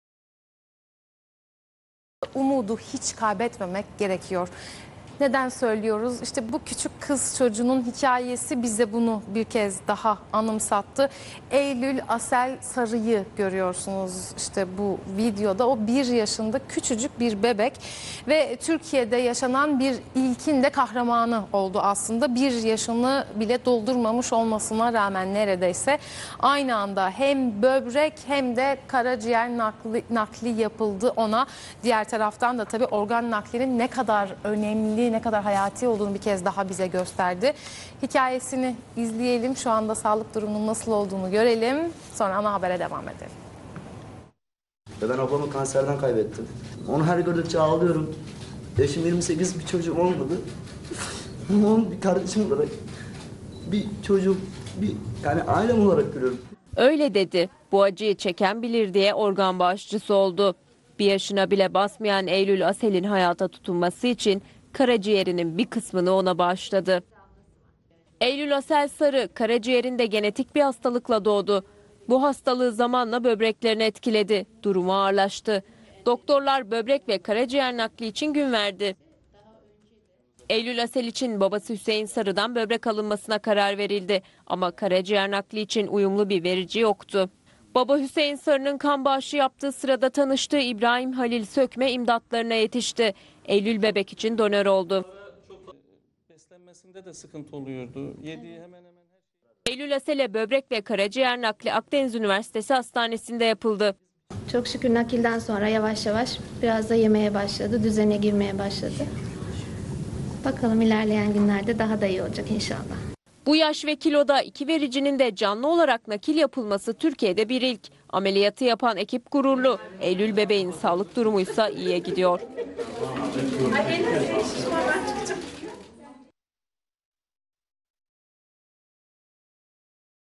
Türkiye'de Bir İlk
Akdeniz Üniversitesi Hastanesi bir ilke daha imza attı. 1 yaşındaki Eylül Akdeniz Üniversitesi Hastanesi’nde gerçekleştirilen  karaciğer ve böbrek nakli ile hayata tutundu.
Akdeniz Üniversitesi Hastanesi Başhekimi Prof. Dr. Bülent Aydınlı dünya üzerinde bu yaş ve kiloda hem karaciğer hem de böbreğin, iki canlı vericiden nakil yapılmasının çok nadir olduğunu belirterek ekibi adına gururlu olduklarını dile getirdi.